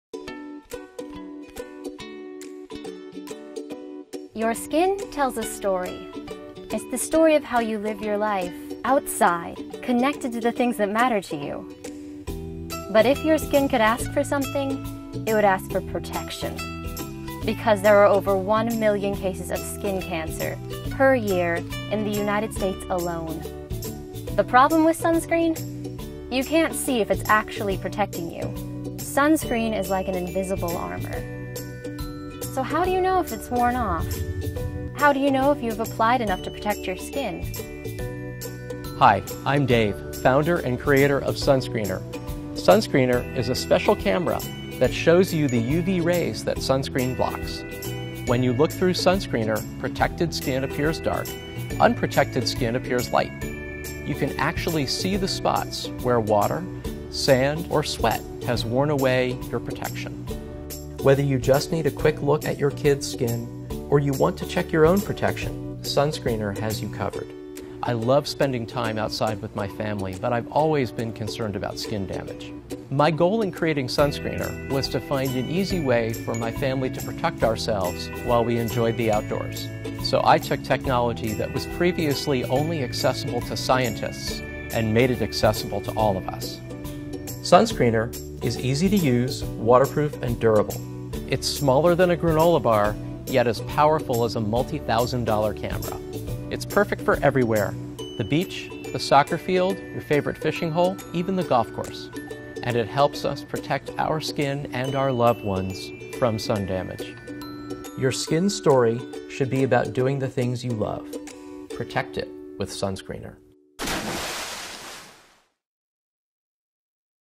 Sunscreenr
Das Gute und gleichzeitig auch das Problem an Sonnencremes - man sieht diese nach dem Auftragen nicht mehr.

Sunscreenr ist eine kleine UV Camera, welche den Sonnenschutz auf deiner Haut sichtbar macht.

So kannst du jederzeit bei dir oder deiner Familie prüfen, ob noch genügend Schutz gegen die UV-Strahlen vorhanden ist.

Die Idee ist nicht schlecht - und das Projekt wurde bereits im letzten Jahr erfolgreich über Kickstarter finanziert. Dennoch warten die Unterstützer noch auf ihre Geräte.

Sobald diese an die Unterstützer ausgeliefert wurden und Sunscreenr regulär bestellt werden kann, informieren wir dich gerne.

Täglich neue coole Gadgets, Trends & Angebote findest du in der Upcoming App.